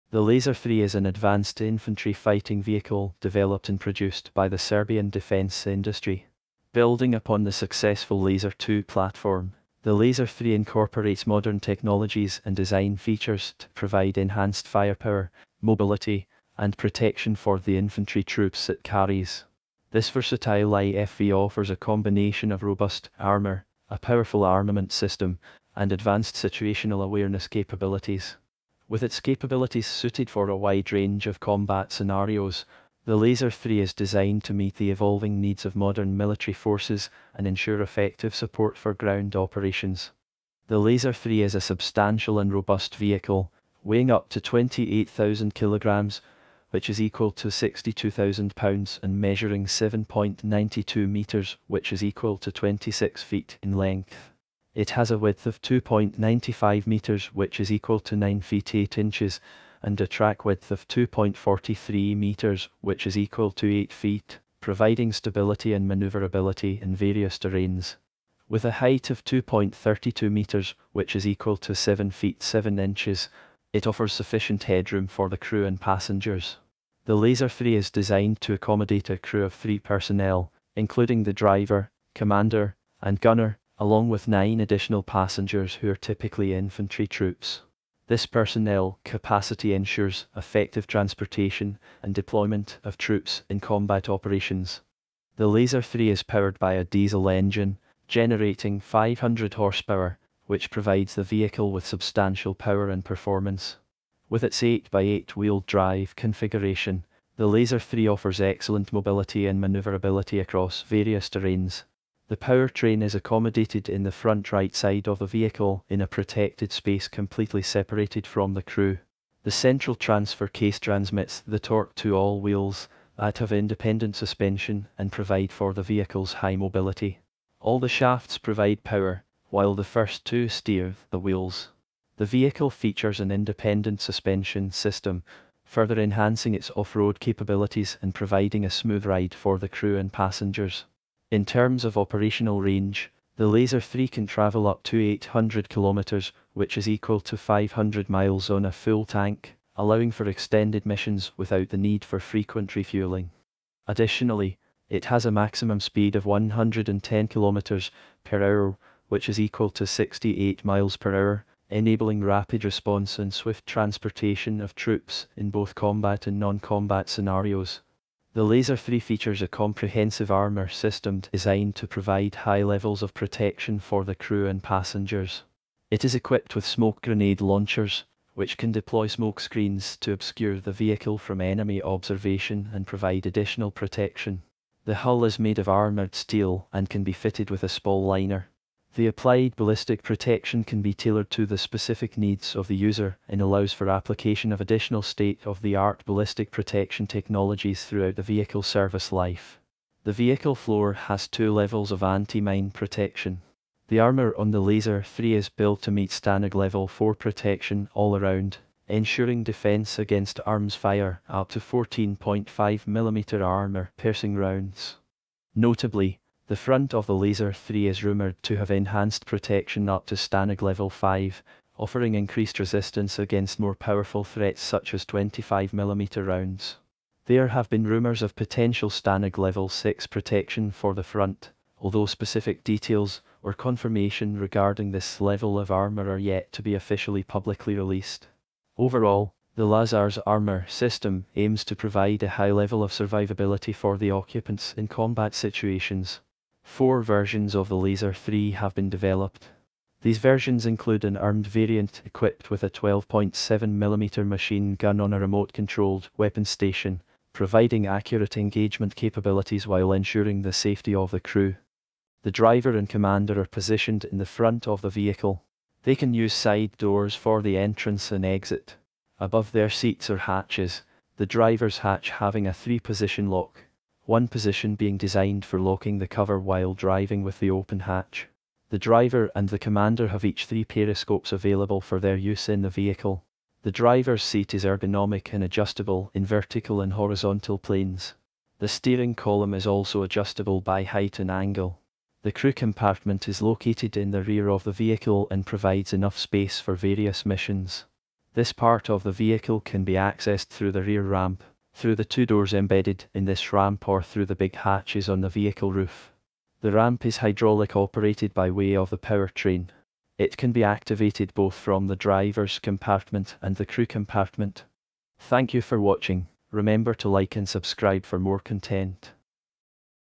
Lazar 3 Infantry Fighting Vehicle (Serbia)
This educational video provides a comprehensive overview of the Lazar 3, an infantry fighting vehicle (IFV). It explores the vehicle's features, capabilities, and historical significance. The video covers the vehicle's origins, development, purpose, and role on the battlefield. It discusses the Lazar 3's mobility, protection, firepower, and communication systems. Archival footage and real-life examples showcase the vehicle in action, highlighting its effectiveness and versatility. The video aims to expand viewers' knowledge and appreciation of this iconic IFV while emphasizing that it is for educational purposes only and does not endorse any specific military or political agenda.